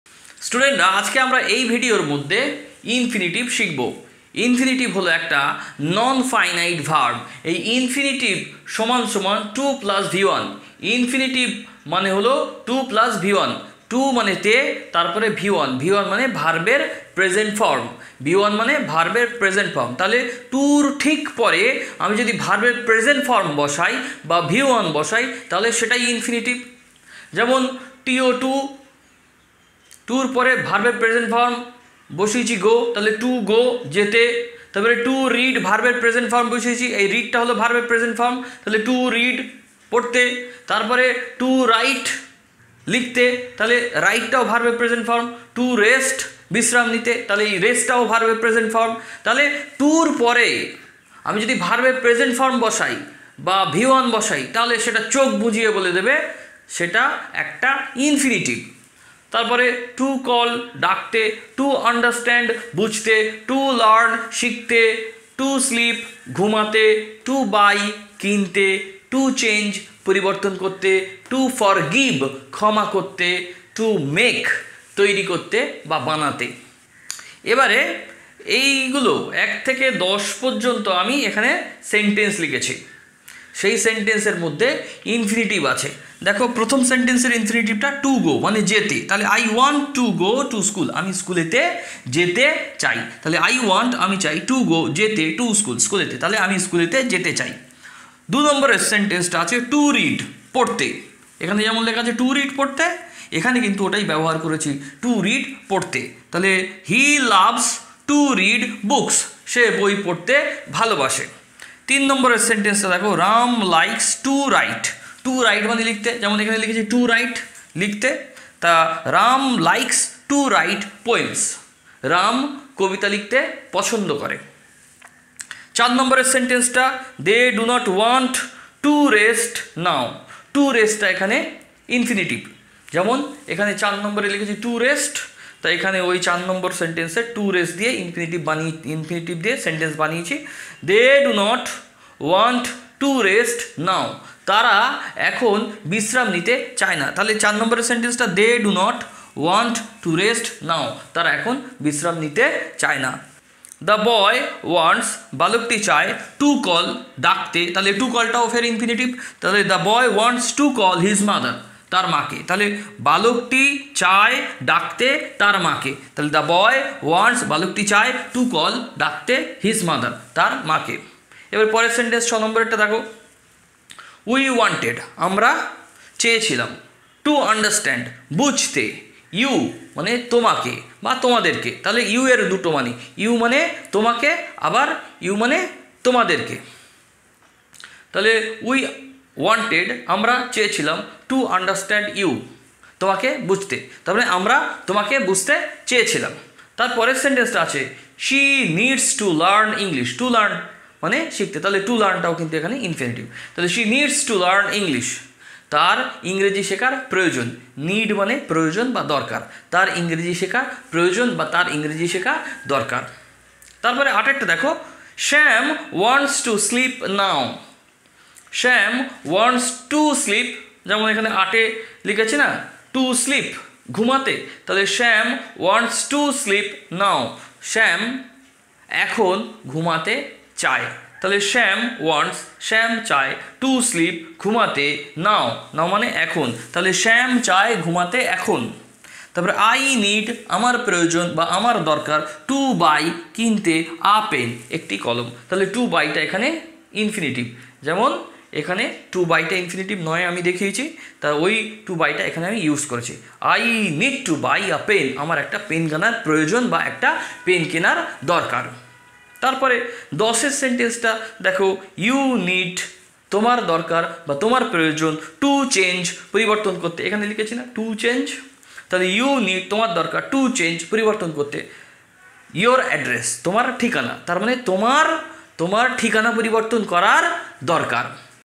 ইংরেজি তে infinitive কি?😱😱🔥 এটি কখন এবং কিভাবে ব্যবহার করতে হয়।
English Point

Wasim Akram Halder